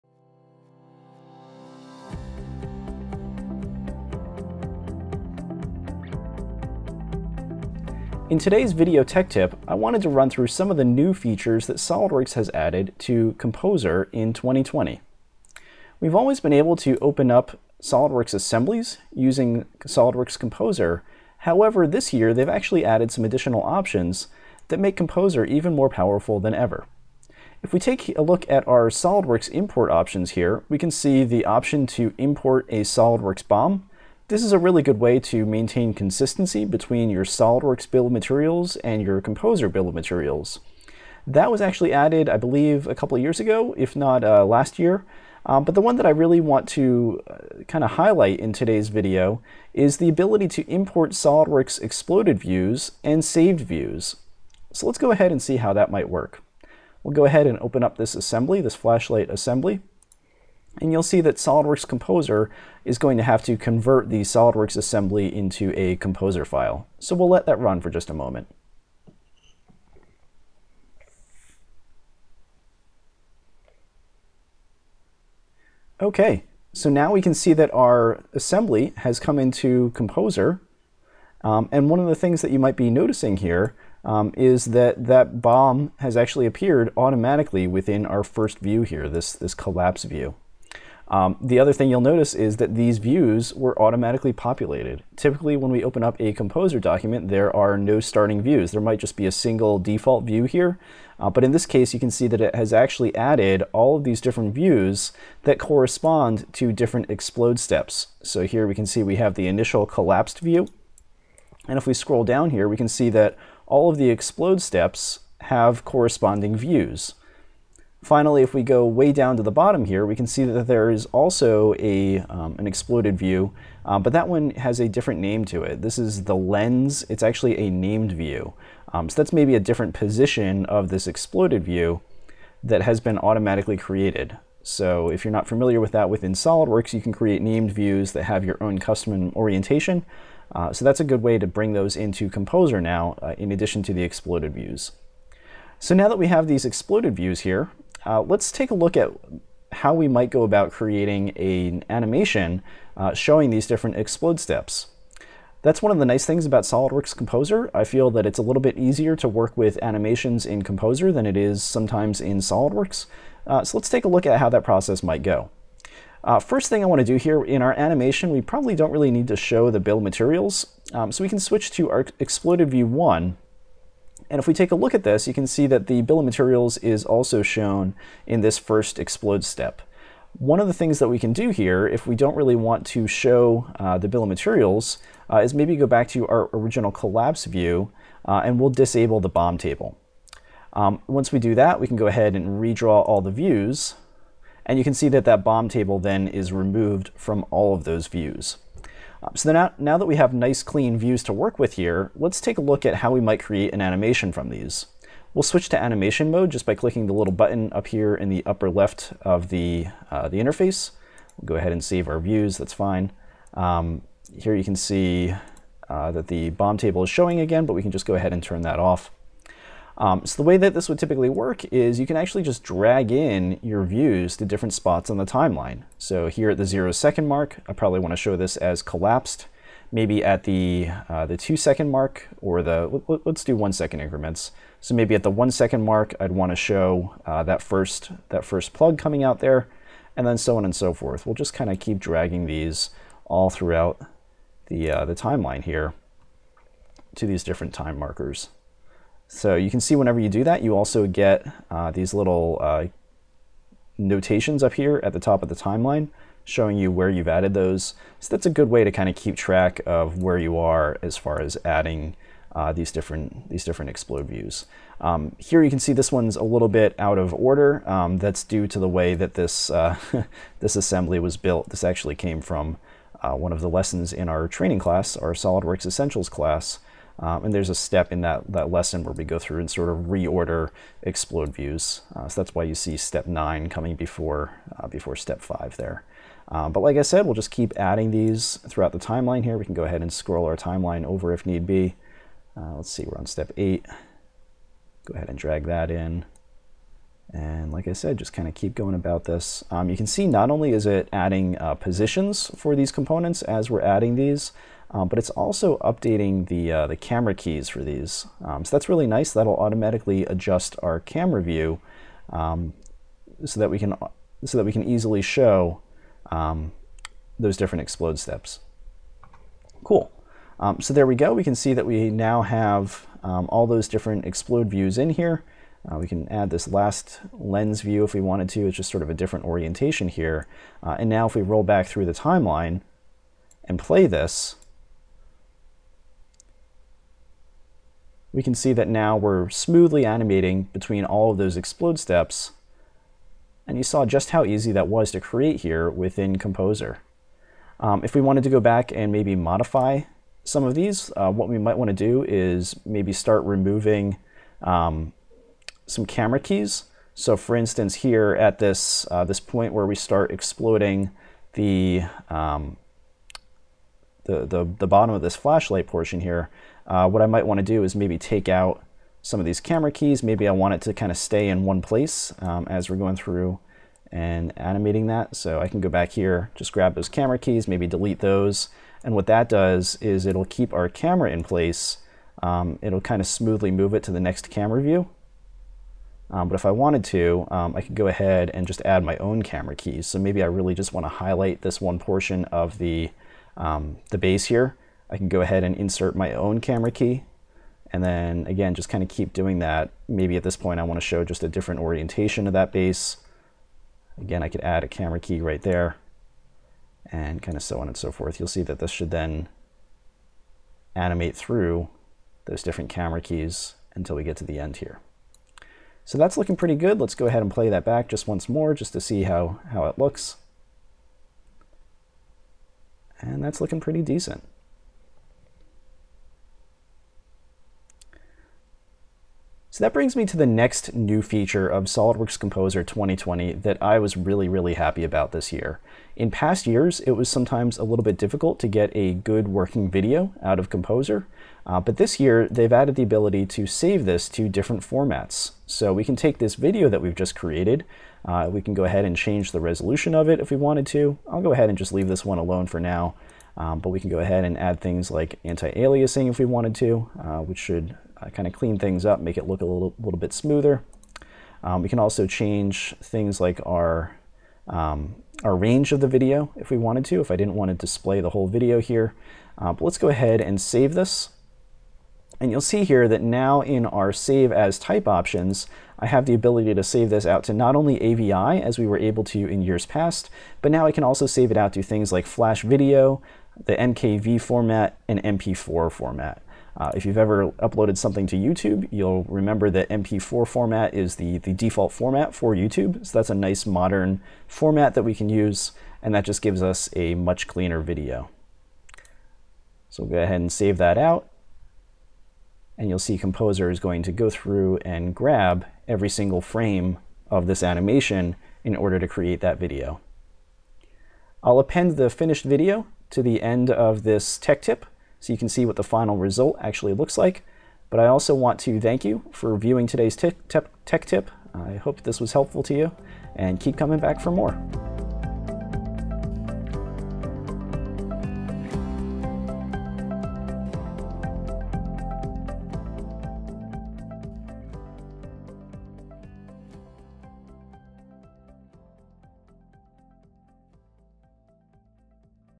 SOLIDWORKS Tech Tip: Import Exploded Views and New Video Formats with SOLIDWORKS Composer 2020
SOLIDWORKS has added more options that make Composer more powerful than ever. Join us as TriMech Application Engineer, Mike Souders, runs through some of the new features SOLIDWORKS has added to Composer in 2020.